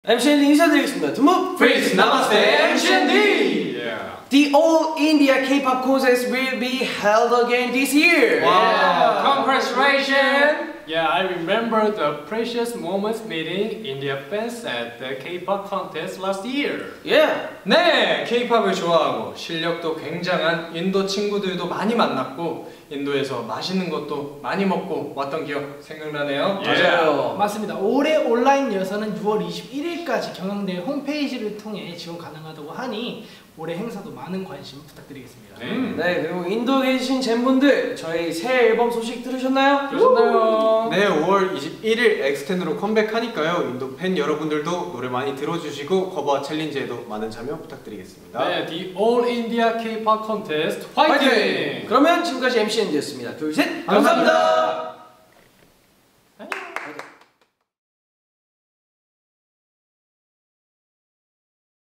MCND wishes the participants of the All India K-Pop Contest 2024 presented by LG all the very best!
Look who has come to greet us all!!🤩

📌With passion and power, the online rounds of the Contest are set to begin from 17th May and are to end on 21st June. With vibrance in your voice and sparkle on your feet, the online rounds would be conducted on the LG website.

The impressive magnitude of the contest would be marking the grandest experience of all time!✨